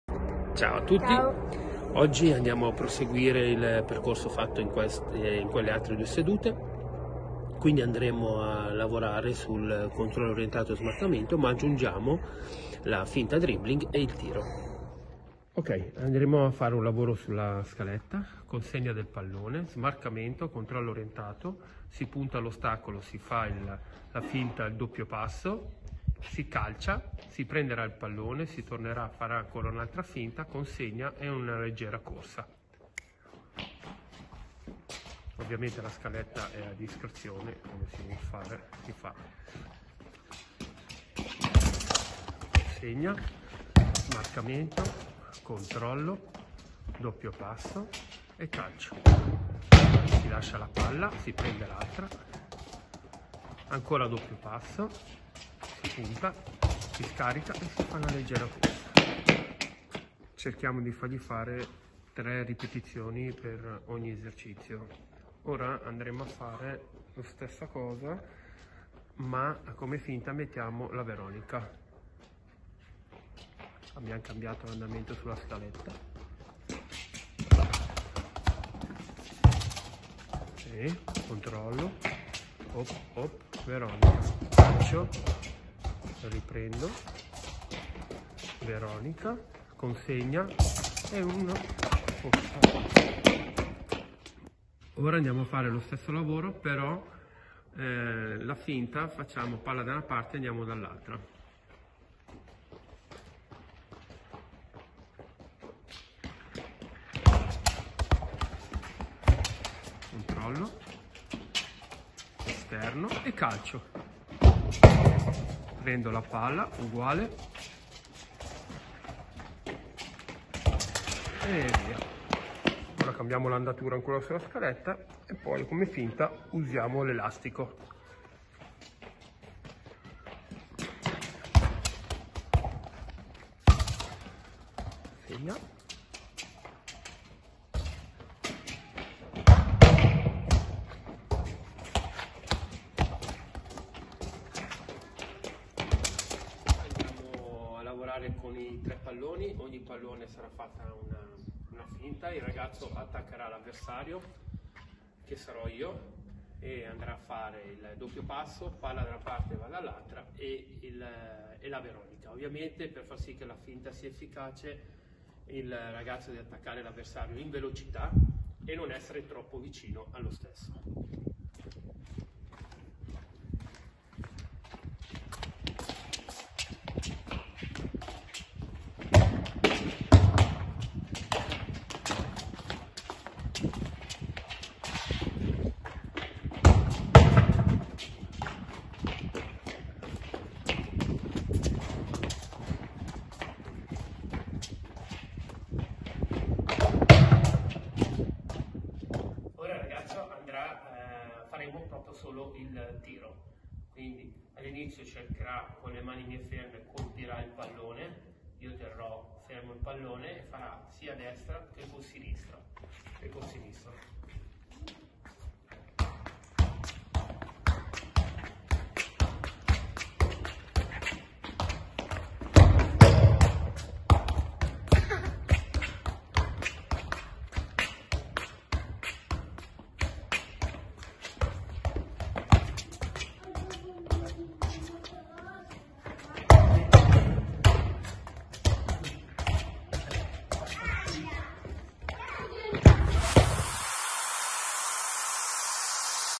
Josè la Cagnina - Controllo orientato e smarcamento (2)
Il tecnico Udinese Academy prosegue la progressione del lavoro sul controllo orientato e lo smarcamento.
Mister Jose La Cagnina propone una esercitazione con diverse varianti per migliorare anche coordinazione, trasmissione e tiro.

In tali proposte inoltre, La Cagnina, aggiunge diversi tipi di finte applicabili poi nel duello 1vs1.